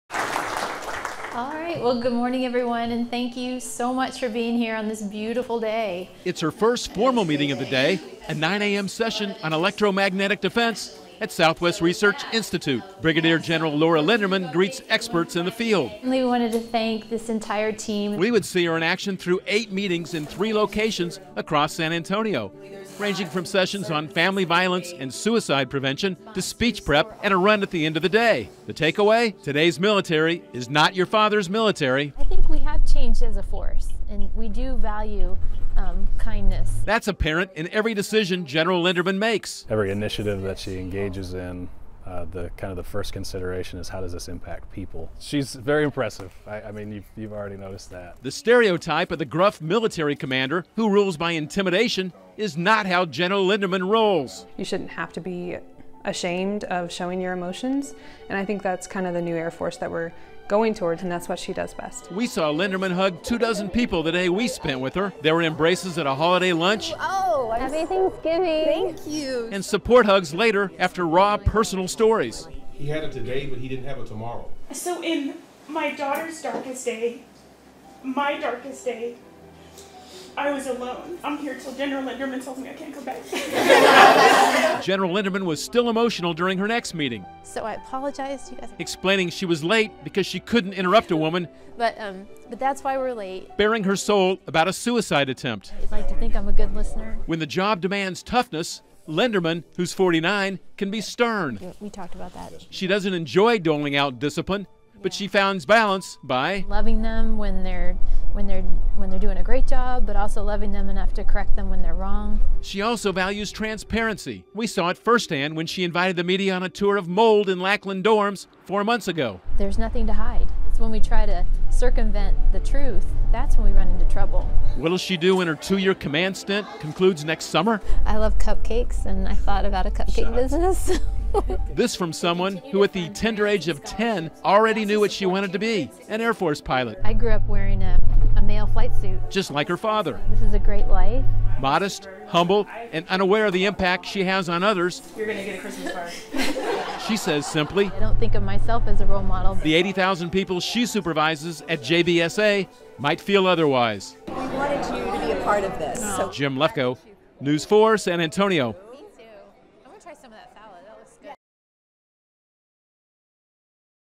Gen Lenderman
Day in the life of Gen Lenderman USAF